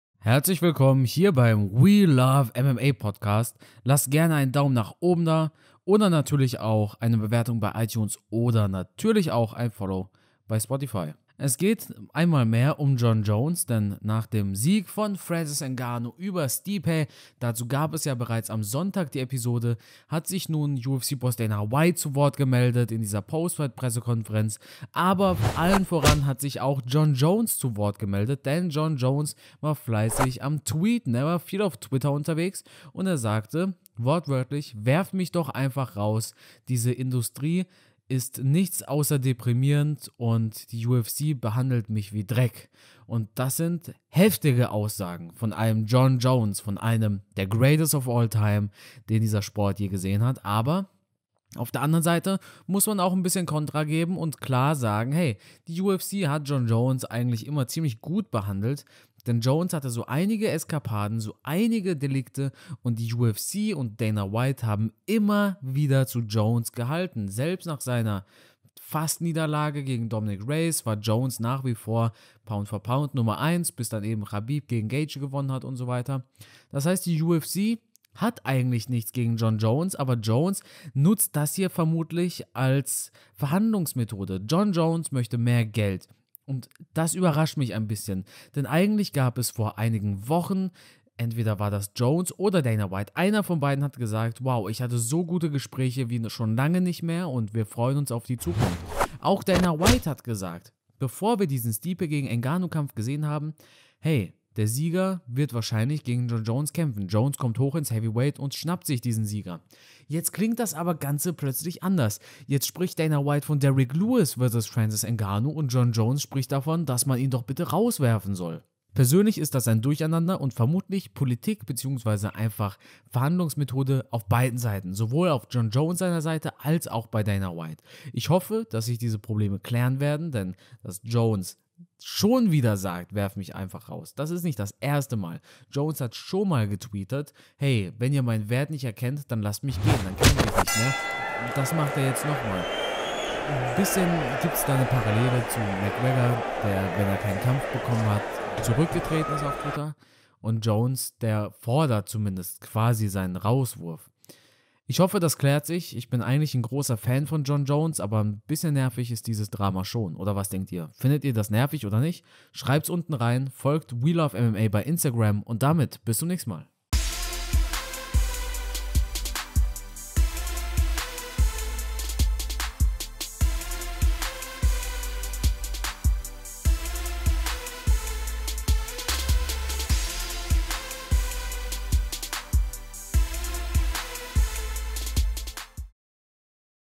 News: Jon Jones im Streit mit der UFC!
Jon Jones fordert seinen Rauswurf. Der Streit mit der UFC droht zu eskalieren - was ist da los?